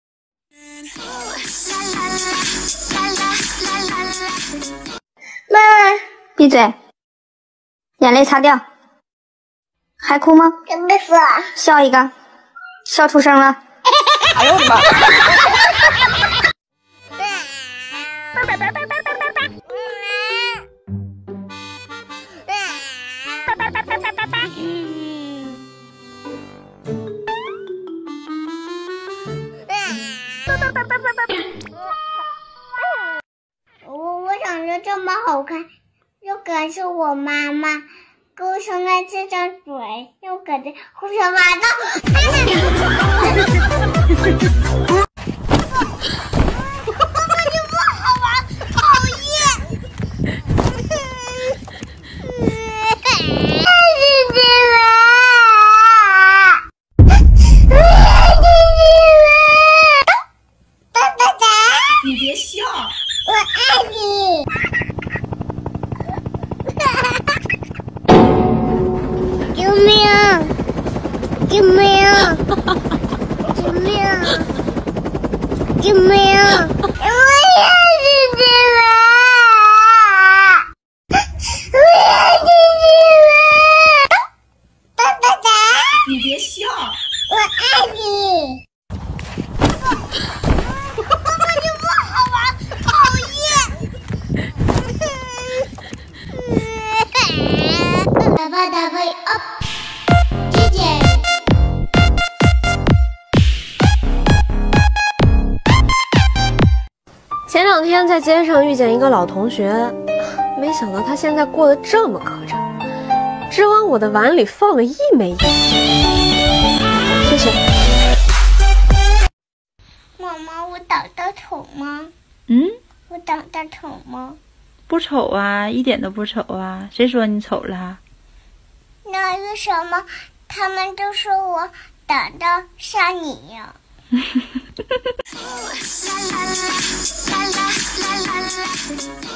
Super Cute Bunny Rabbit TikTok |Jeffrey mensen Green Rabbit English subtitles 44
Hello everyone i have edit this Compilation video with bunny rabbit tiktok and translation in English, bunny rabbit is so cute and funny hope you like this video let me know in comments

1) bunny rabbit is crying and his mom says clean your tears and smile
2) bunny rabbit is crying and someone is annoying the bunny rabbit
3) bunny rabbit is crying he want to go out to play his mom gives him a lollipop and he is happy know
4) bunny rabbit is riding a cow and singing and dancing
5) bunny rabbit is surprise how purely her friends lives because he gives just a penny to her bowl
6)bunny rabbit is sad he thinks he looks ugly

Note
Im not own this video credits going to green rabbit Tiktok

All the credits going to original users